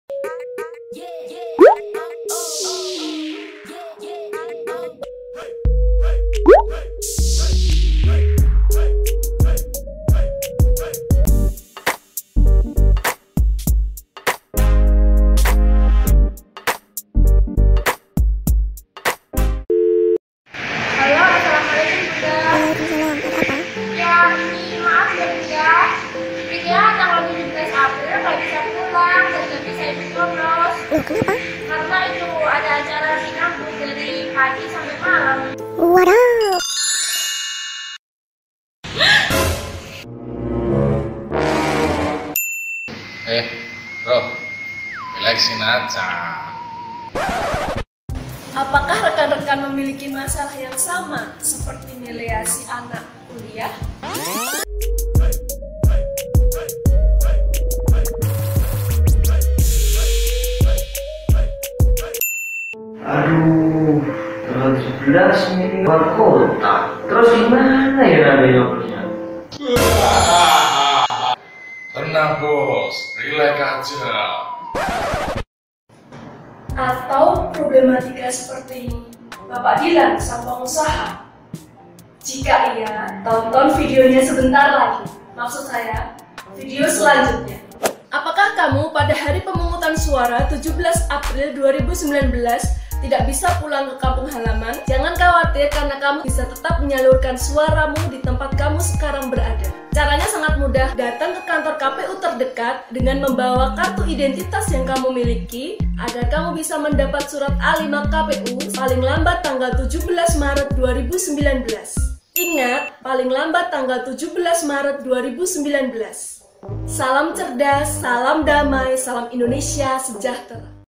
DILAN & MILEA PINDAH MEMILIH | PEMILU 2019 | PART 2
HALO GUYS, APA KAMU JUGA MENGALAMI KEGALAUAN SEPERTI DILAN & MILEA?
TONTON VIDEO INI UNTUK MENEMUKAN SOLUSINYA.
JIKA SUDAH BAHAGIA, SHARE YAH, AGAR TEMAN GALAU MU YANG LAIN BISA TERTAWA RIANG GEMBIRA.
-SALAM CERDAS, SALAM DAMAI, SALAM INDONESIA SEJAHTERA-